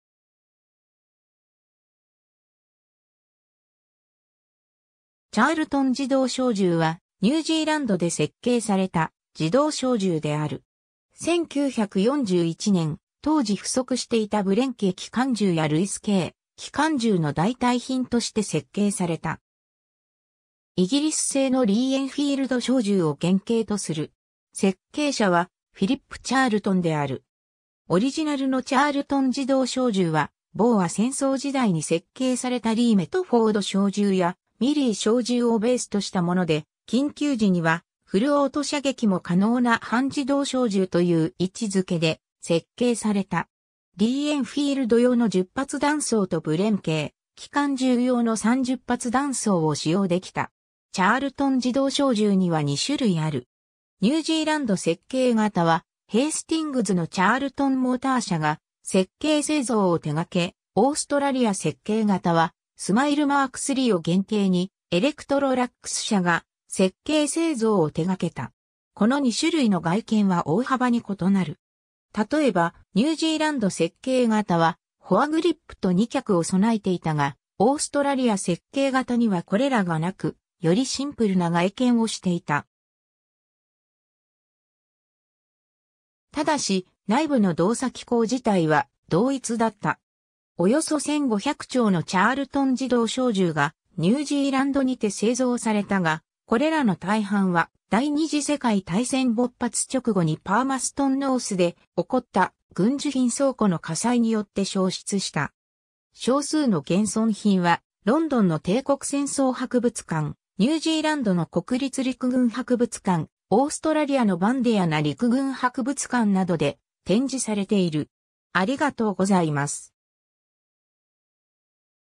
チャールトン自動小銃
#7.7mm銃
チャールトン自動小銃（Charlton Automatic Rifle）は、ニュージーランドで設計された自動小銃である。
1941年、当時不足していたブレン軽機関銃やルイス軽機関銃の代替品として設計された。
イギリス製のリー・エンフィールド小銃を原型とする。
 設計者はフィリップ・チャールトン（Philip Charlton）である。
 オリジナルのチャールトン自動小銃は、ボーア戦争時代に設計されたリー・メトフォード小銃やMLE小銃をベースとしたもので、「緊急時にはフルオート射撃も可能な半自動小銃」という位置づけで設計された。
リー・エンフィールド用の10発弾倉とブレン軽機関銃用の30発弾倉を使用できた。
 チャールトン自動小銃には2種類ある。
ニュージーランド設計型はヘイスティングズのチャールトン・モーター社（Charlton Motor Workshops）が設計製造を手がけ、オーストラリア設計型はSMLE Mk III*を原型にエレクトロラックス社が設計製造を手がけた。
この2種類の外見は大幅に異なる。
例えばニュージーランド設計型はフォアグリップと二脚を備えていたが、オーストラリア設計型にはこれらが無く、よりシンプルな外見をしていた。
ただし、内部の動作機構自体は同一だった。
 およそ1,500丁のチャールトン自動小銃がニュージーランドにて製造されたが、これらの大半は第二次世界大戦勃発直後にパーマストンノースで起こった軍需品倉庫の火災によって焼失した。
少数の現存品はロンドンの帝国戦争博物館、ニュージーランドの国立陸軍博物館、オーストラリアのバンディアナ陸軍博物館（Army Museum Bandiana）などで展示されている。